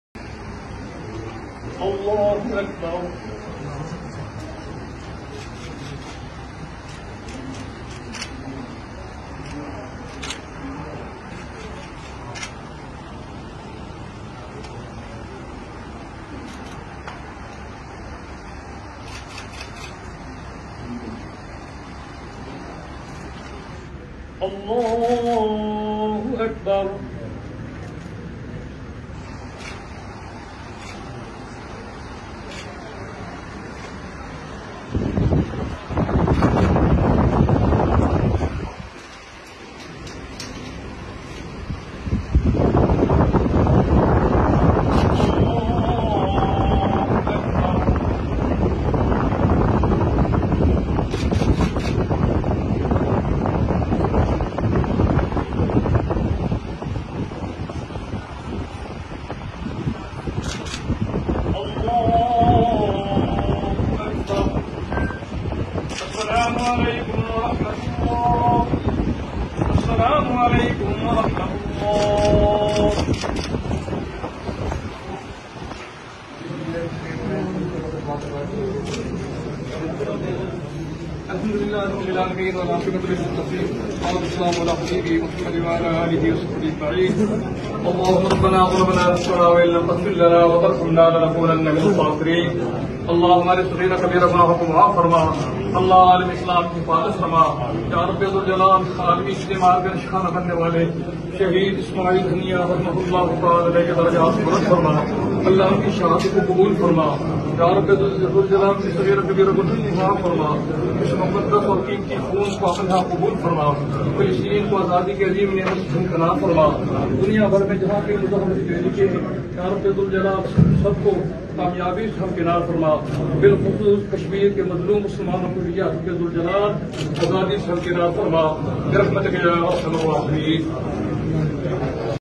All parlimentairain together namaz ba jamat performing including P.M
MashaAllah all members of national assembly performing namaz ba jamat